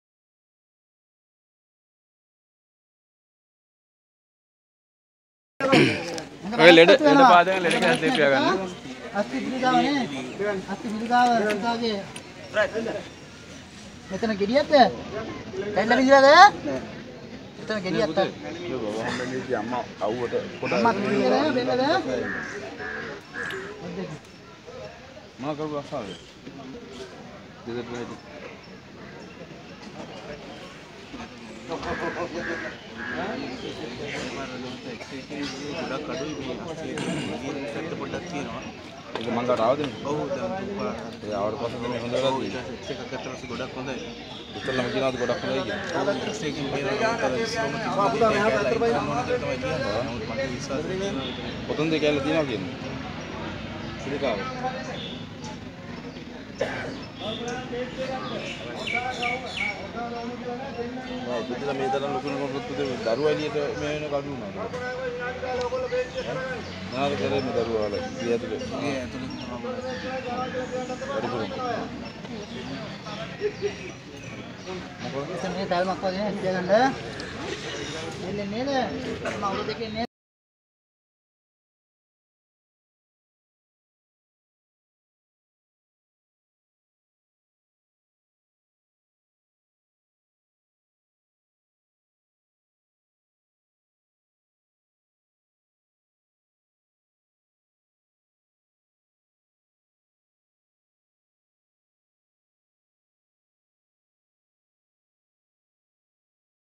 අස්ථි පිළිකාවෙන් පෙළුන දරුවා එක්දිනක ප්‍රතිකාරයෙන්ම සුව වූ බව වෛද්‍ය පරීක්ෂණ මගින් තහවුරු වෙයි.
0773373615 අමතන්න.